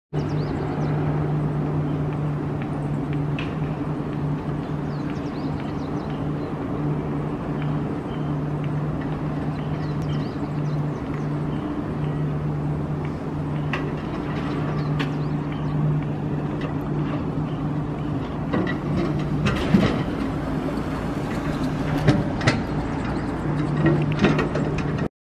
スカイレールみどり坂線みどり中街～みどり口間（１）
みどり中街～みどり口間を走る200形。